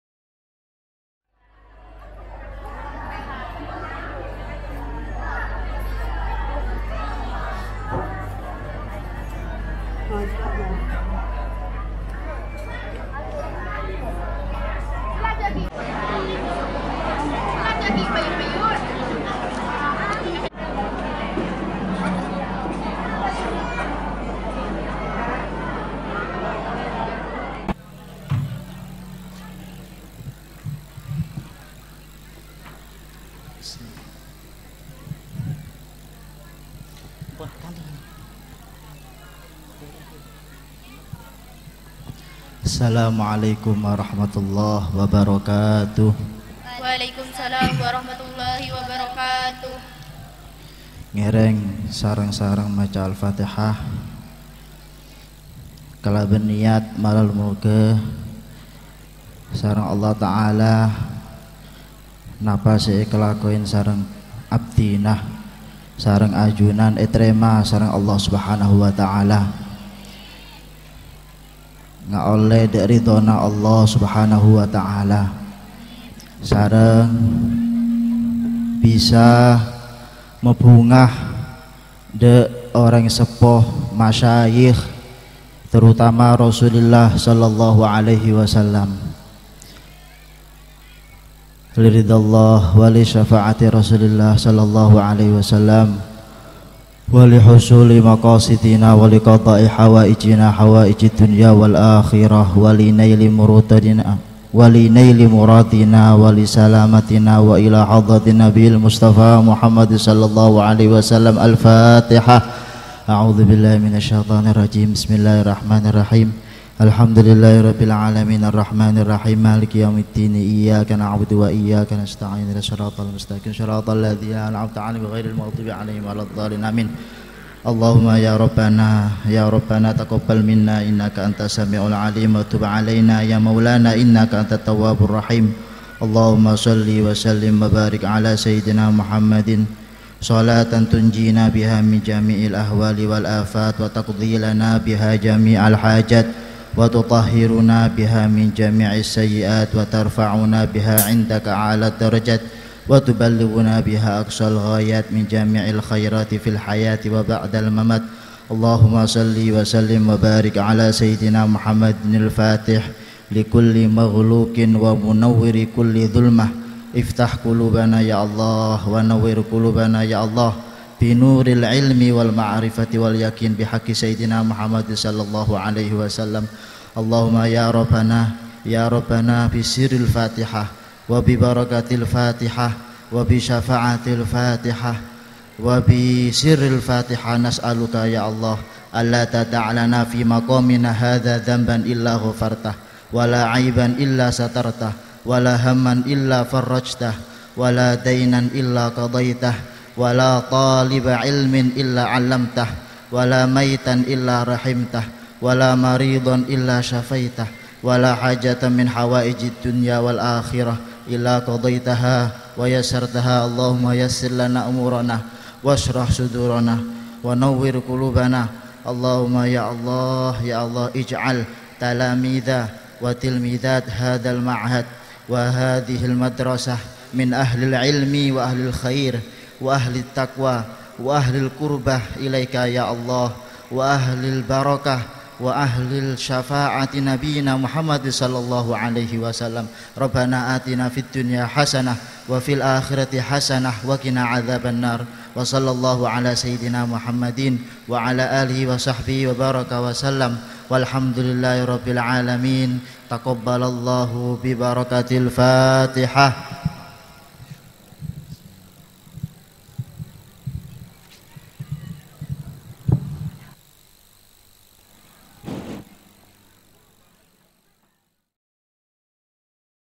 Doa oleh KH. Mas Majdi Tholhah
Wisuda bersama
RA, SMP, SMA Islam AT-TAROQQI TSANI, Pangilen - Sampang Madura
04 Maret 2023